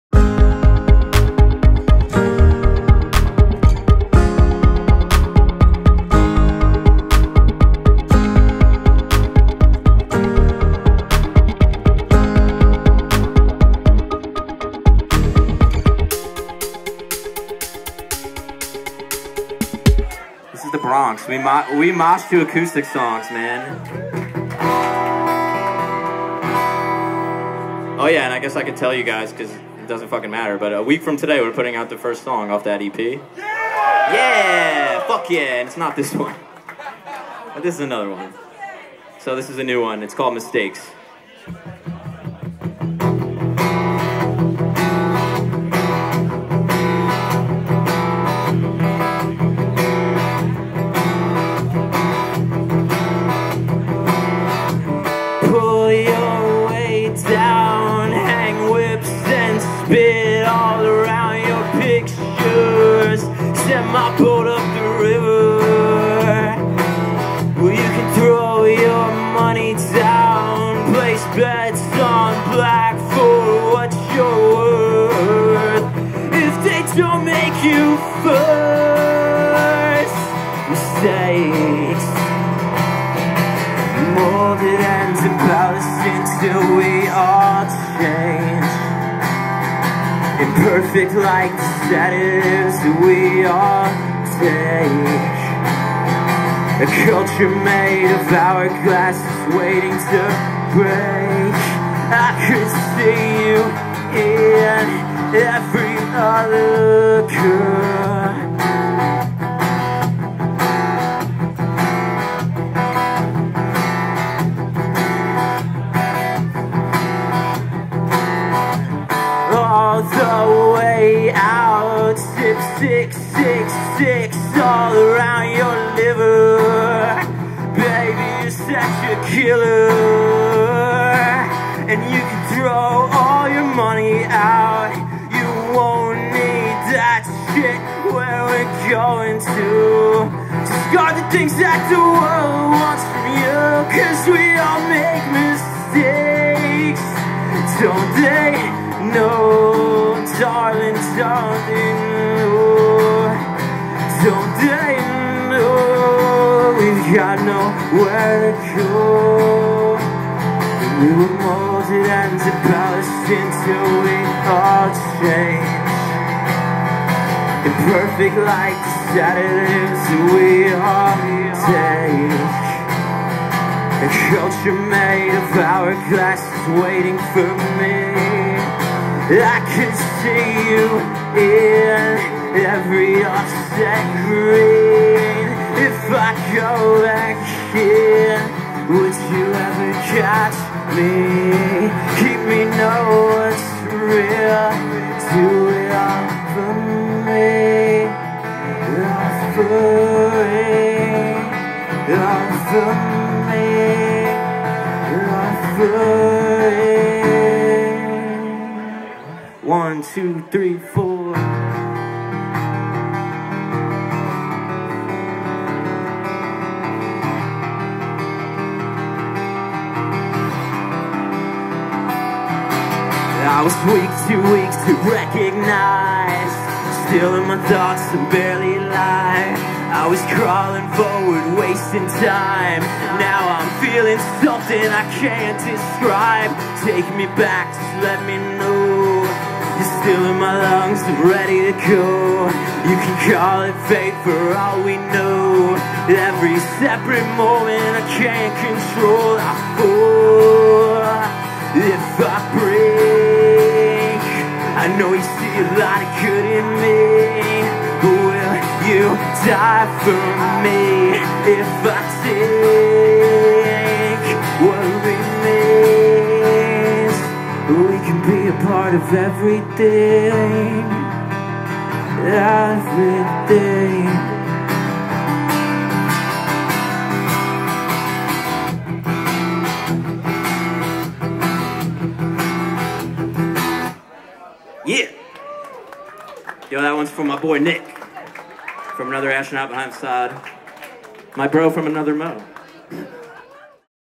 Sean Dowling | Stand Against Hate | Live @ Jimmy Ryan's 9.15.17
On 9.16 we gathered to Stand Against Hate at Jimmy Ryan's in the Bronx, NY. We raised support for Human Rights (ALCU & Peace First and Hurricane Relief (Team Rubicon

VIDEOS by Sweet Lou Photography (YOUR PREFERRED LINK)